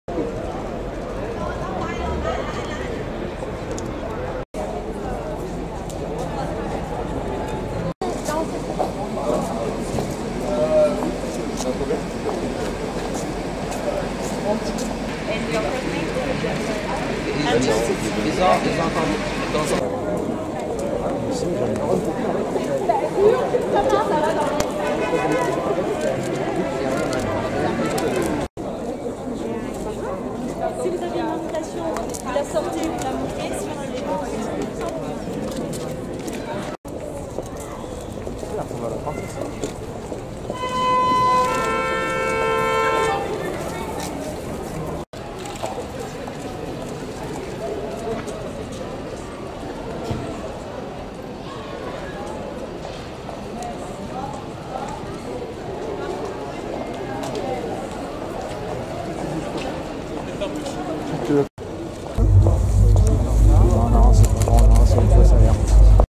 Célia KRYCHOWIAK - JAUNAT and other guests @ show Ralph & Russo Paris 21 january 2019 Fashion Week
Célia JAUNAT wife of Grzegorz KRYCHOWIAK and more vip's and guests @ show Ralph & Russo Paris 21 january 2019 Fashion Week